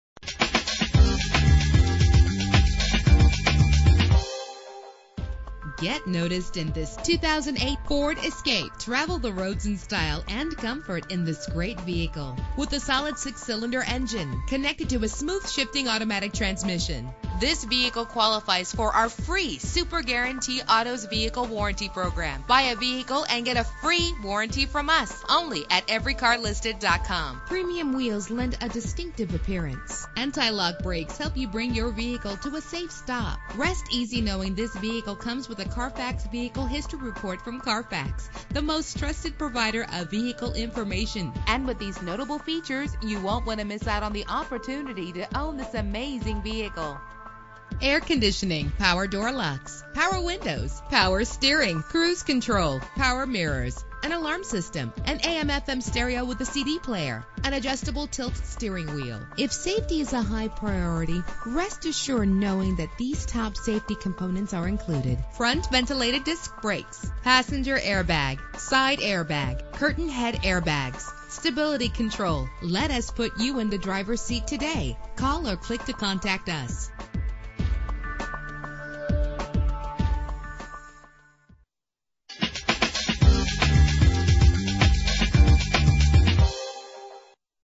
2008 Ford Escape - Thornton CO
Year: 2008
Make: Ford
Model: Escape
Trim: Limited
Engine: 3.0 liter 6 cylinder 24 valve
Transmission: 4-Speed Automatic
Color: Redfire Metallic
Mileage: 29300
Address:
1650 W 104th St